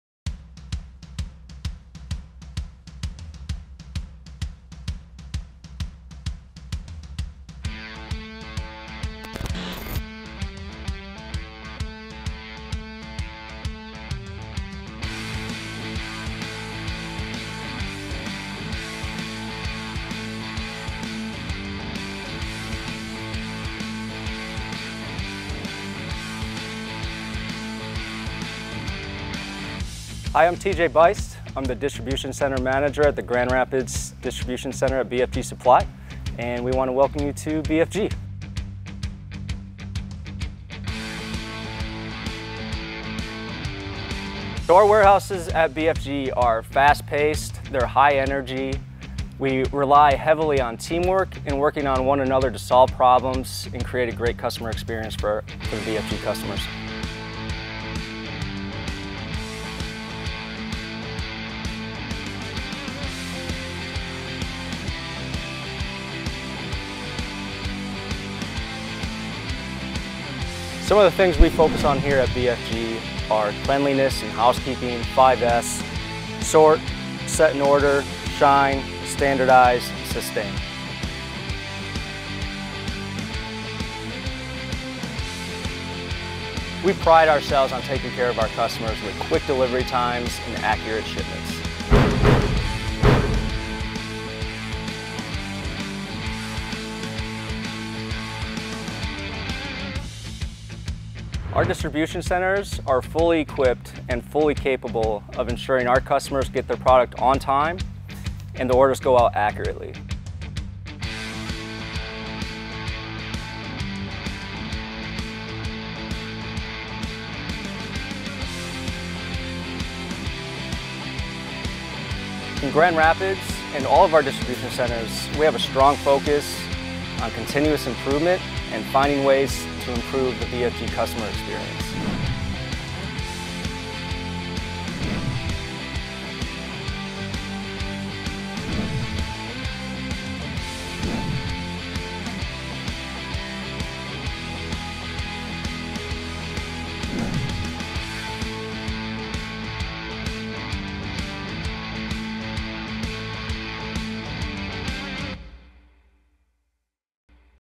BFG Supply Co: Grand Rapids, Michigan
BFG Supply Co., LLC. supplies nursery, garden, and greenhouse products. The Company offers plants, hardgoods, seeds, fertilizer, garden decor, grower chemicals, watering supplies, weed control, and greenhouse covering products. BFG Supply serves lawn and garden industries in the United States.